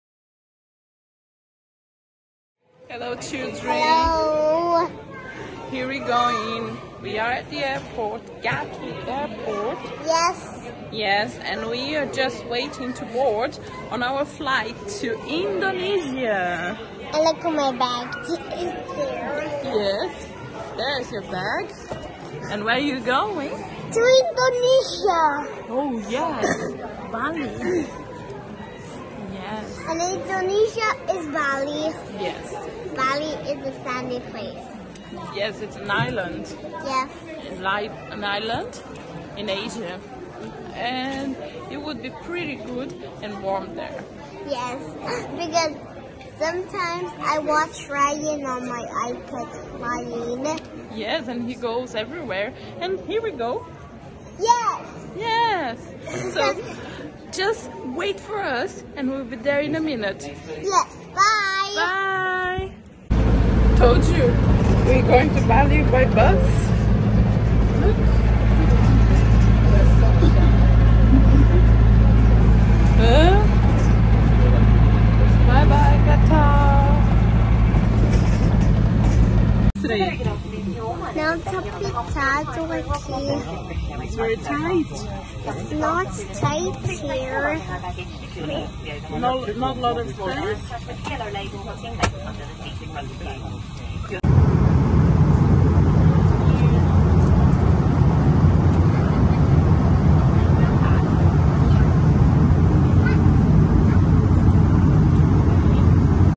Boarding British Airways Flgiht Gatwick to Doha
Mum and I was Boarding British Airways Flight from London Gatwick to Doha.

First sector of our two sector air trip to Bali.